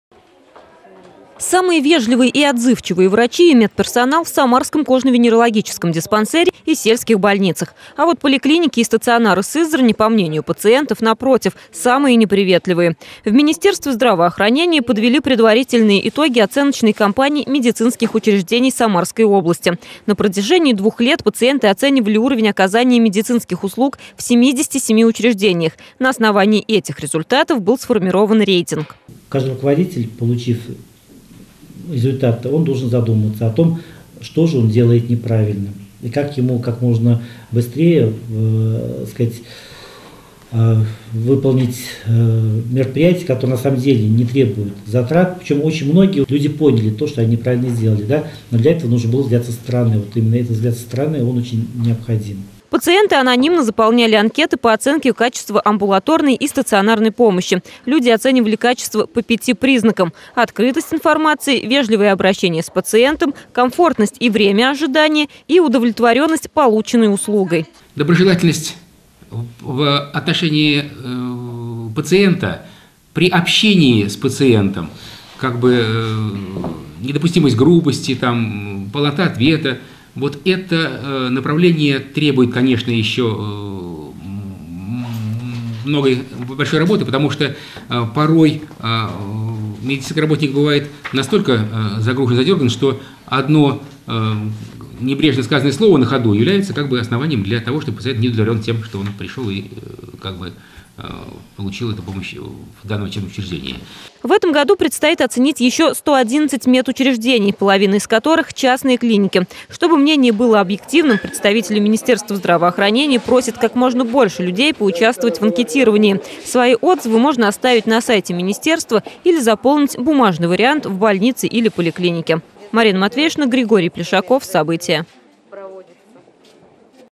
Оценку самарским больницам поставили сами пациенты
Оценку самарским больницам поставили сами пациенты.  В Самаре подвели предварительные итоги независимого исследования. В течение двух лет люди могли оставить отзывы о больницах и поликлиниках, куда они обращаются, и оценить их по нескольким критериям. На основании этих отзывов был составлен рейтинг самых приветливых и комфортных медучреждений.